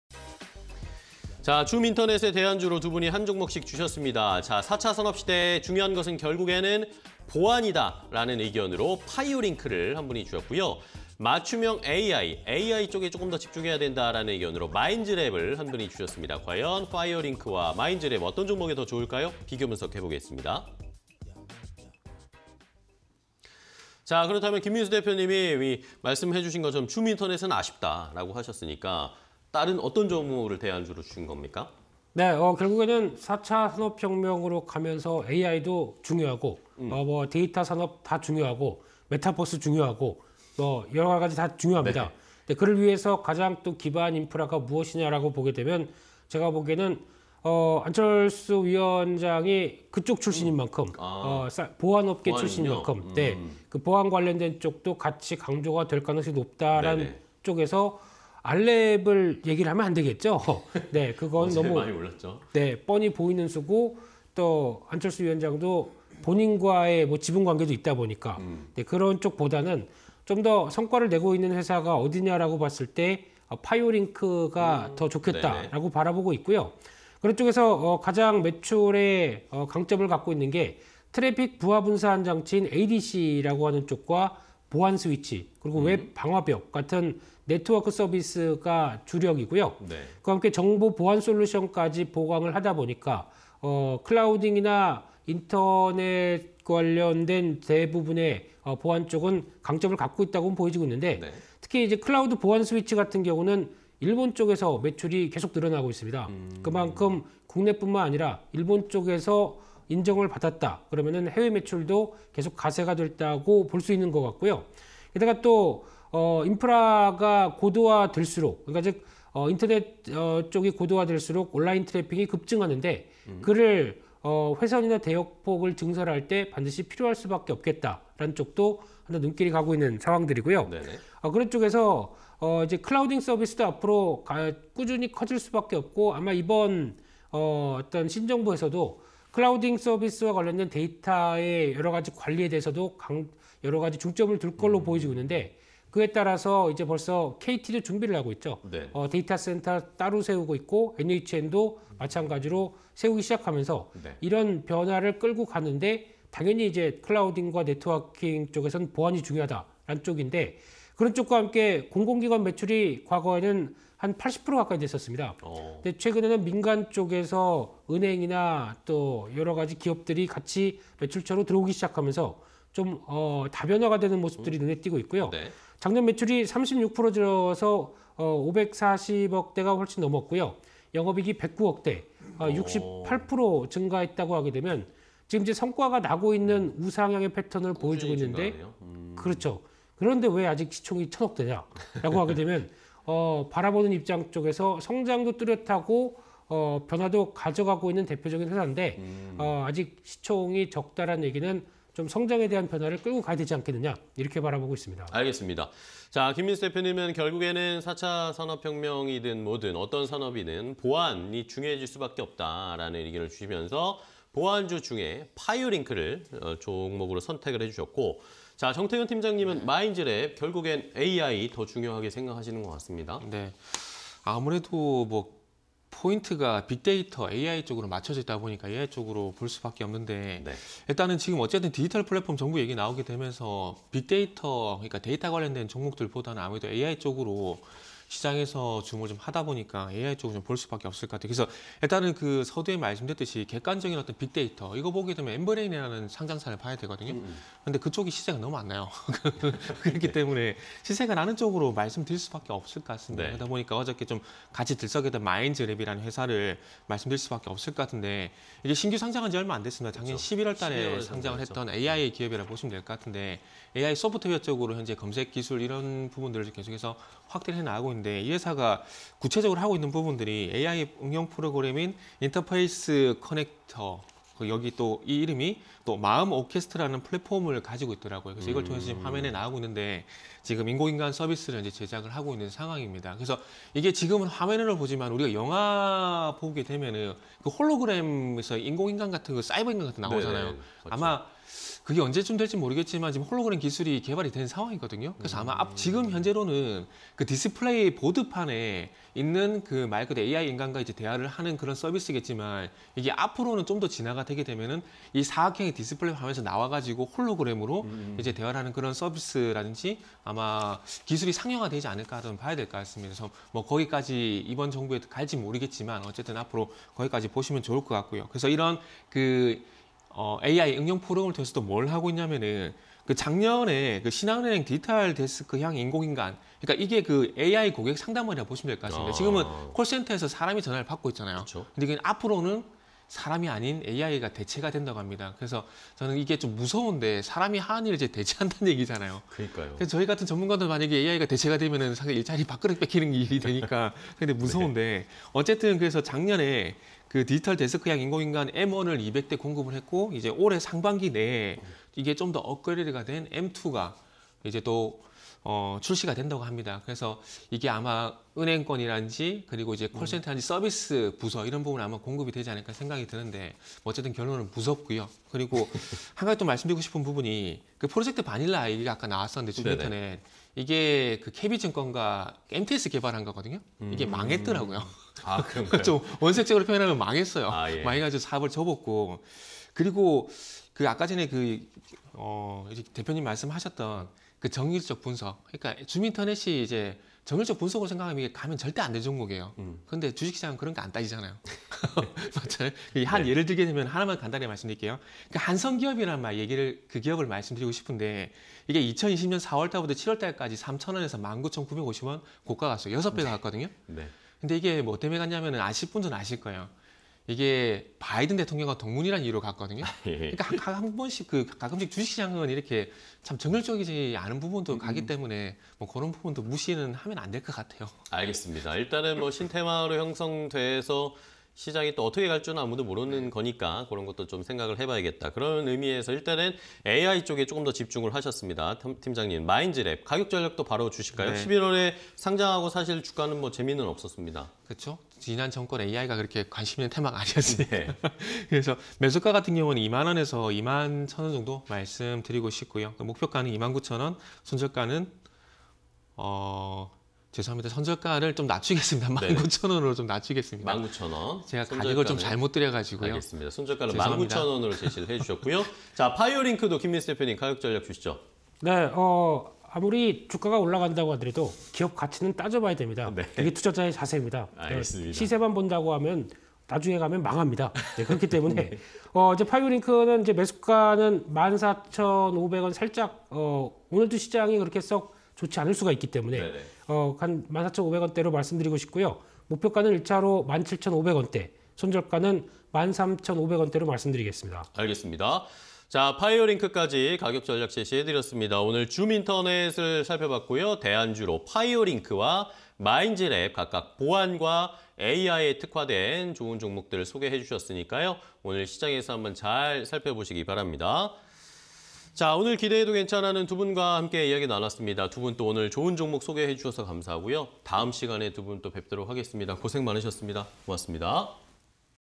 파이오링크... 4차산업시대, 보안의 중요성 VS 마인즈랩... 맞춤형 AI 기업_대안주를 찾아라 (20220315)
- 네트워크 서비스 및 정보 보안 주력
- 국내 및 일본 등 해외 매출 증가
- 온라인 데이터 트래픽 급증에 수혜
- 메타버스 등 신산업 필수 인프라 초점
- 공공기관 매출 80%, 민간 매출처 확대

- 직접 개발한 AI 엔진과 응용 프로그램 보유
- AI 응용 플랫폼 통해 인공인간 서비스 제작
- AI 고객 상담원 등 맞춤형 AI 제작
- 작년 인공인간 'M1' 200대 공급
- 상반기 정확도 개선한 'M2' 출시 예정